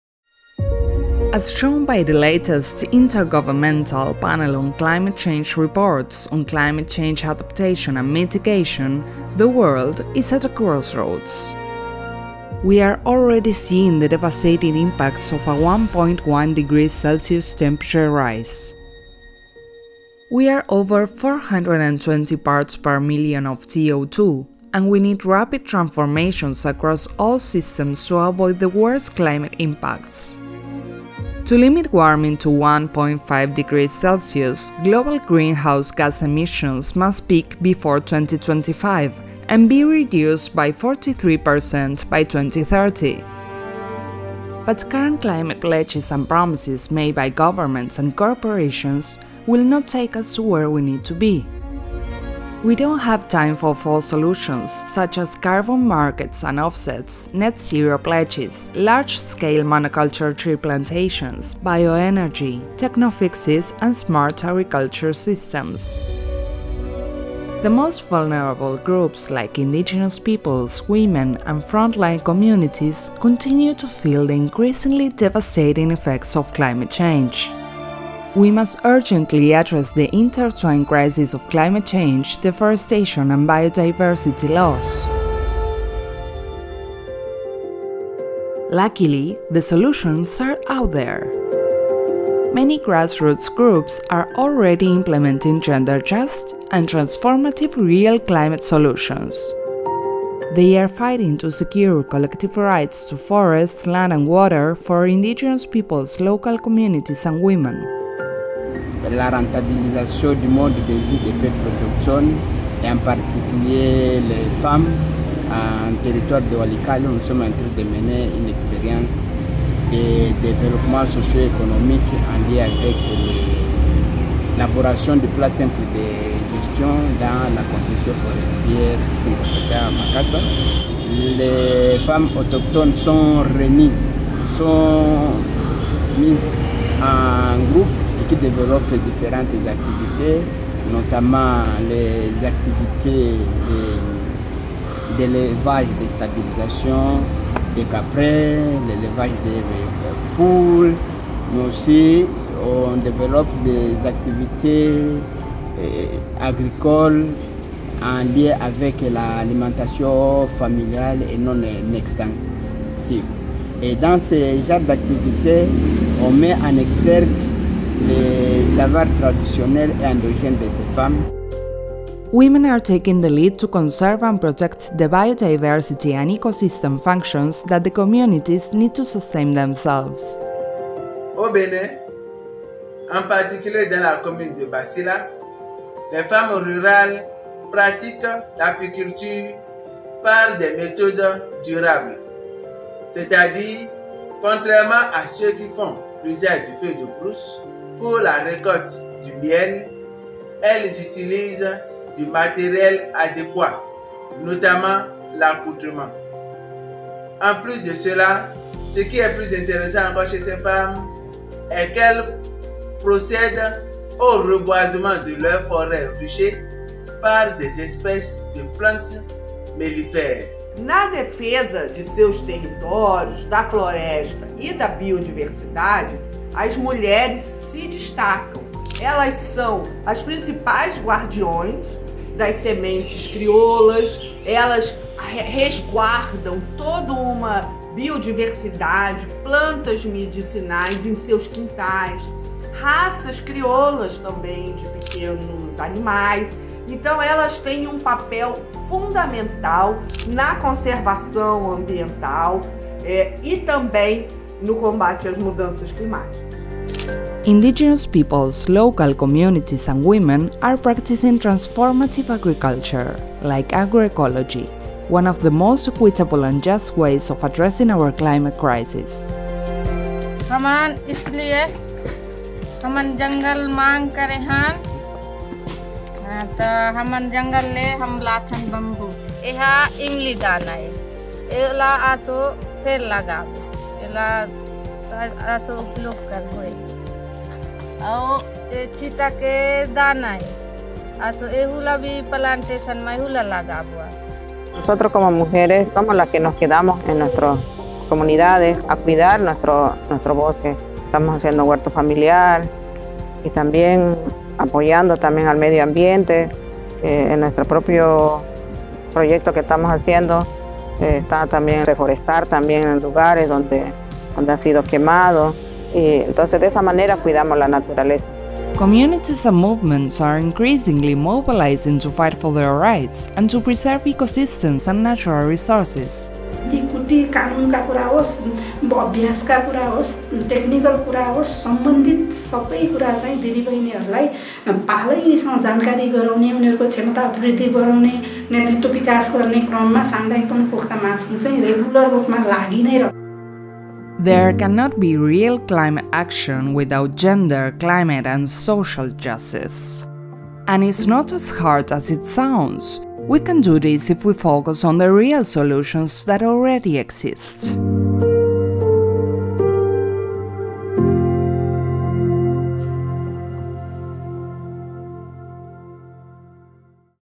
What are the real solutions to the climate change crisis?
Indigenous and local communities around the world are already implenting real solutions to climate change, as opposed to false solutions like geoengineering, carbon trading, carbon offsets, payments for environmental services, REDD, and biodiversity offsets.